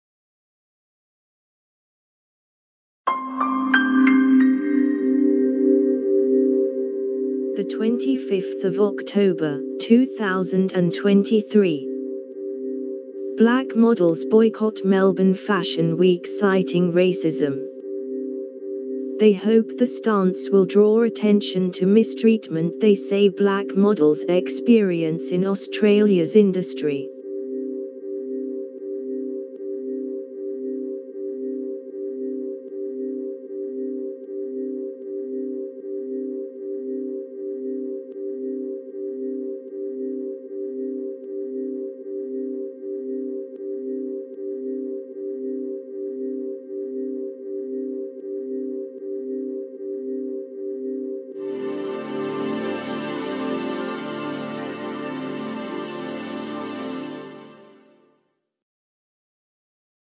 [Latest]Black models boycott Melbourne fashion week citing racism
[Contents]
They hope the stance will draw attention to mistreatment they say black models experience in Australia's industry.